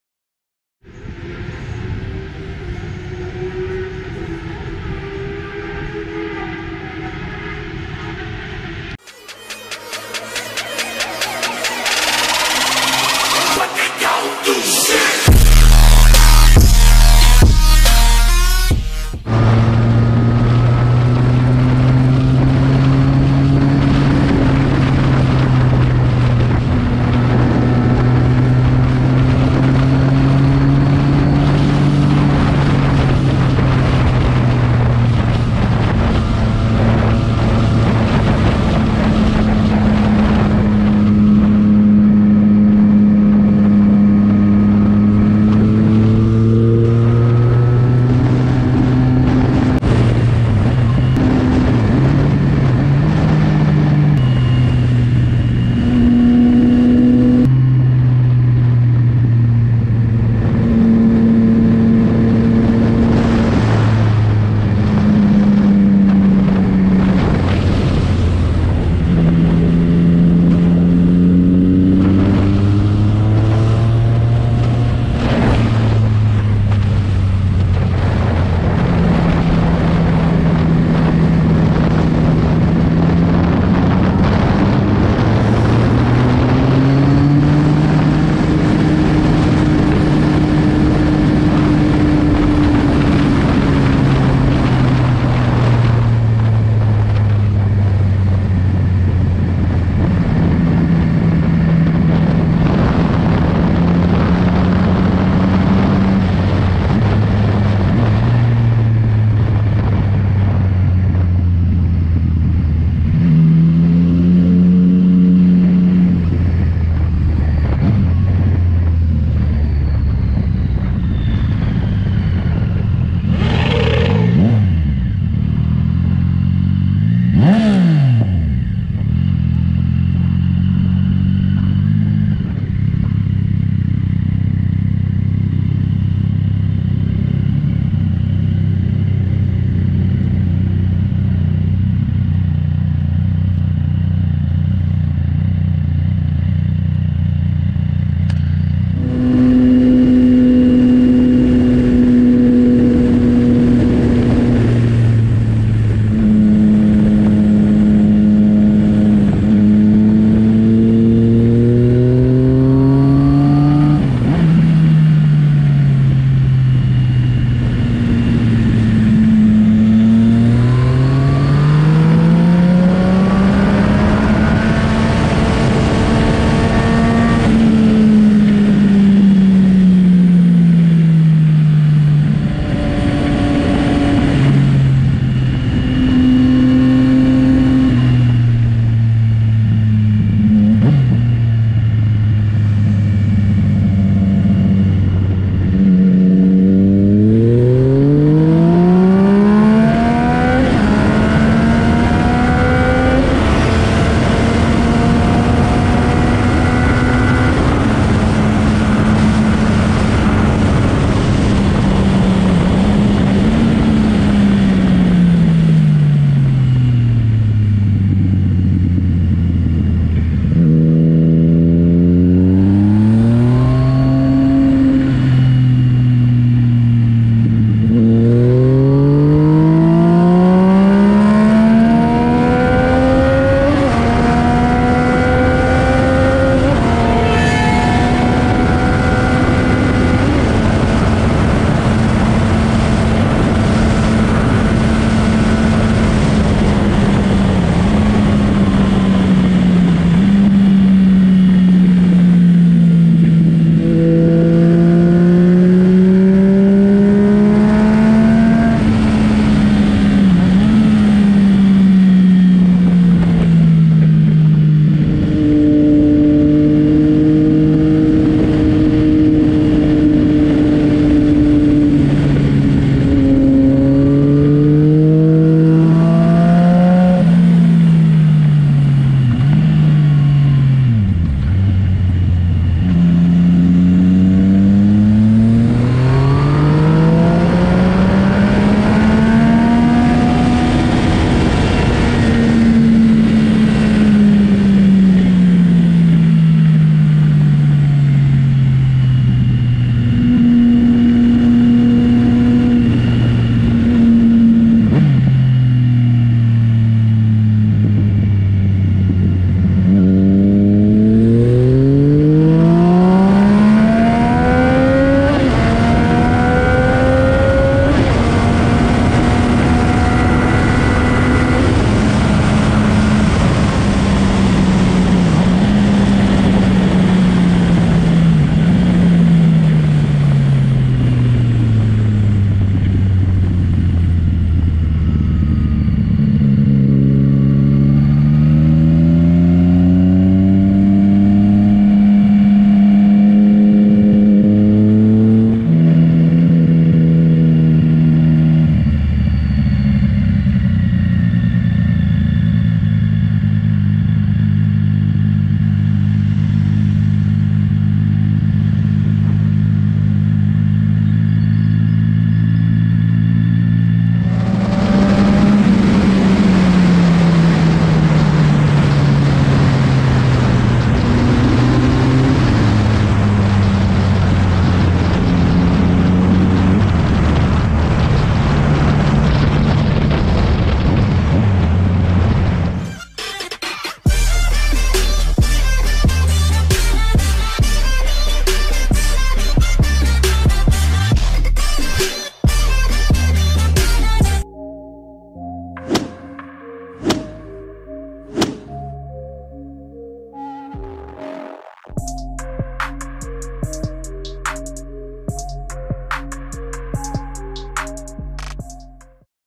MT09 AKRAPOVIC Sound on PAUL RICARD FOR THE VERY FIRST TIME ON TRACK
Deixo-vos aqui um pequeno excerto da 1a e 2a sessões do fim-de-semana e também a minha primeira vez em circuito com a minha Yamaha MT 09, enquanto trabalho a edição dos restantes videos.
Mods: Akrapovic no db killer

JON DIESEL INFO :
Vídeos todas as semanas !
⚠️videos fully edited with iphone 📱